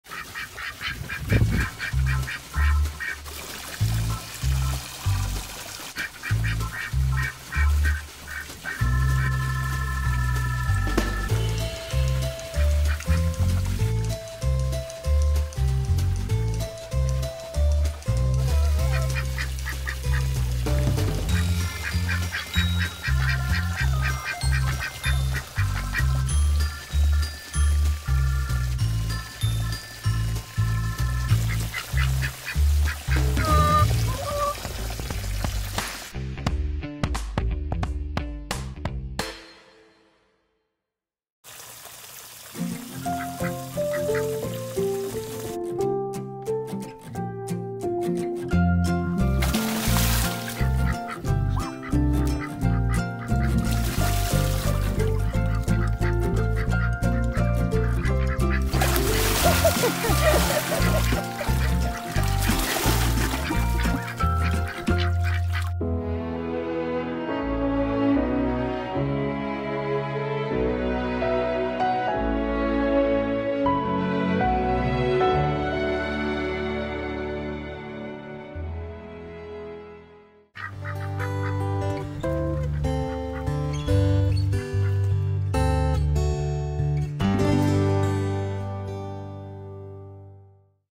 Quackers and Sunflower: Big Pool, Big Fun
Quackers and Sunflower dive into a larger pool and more fun!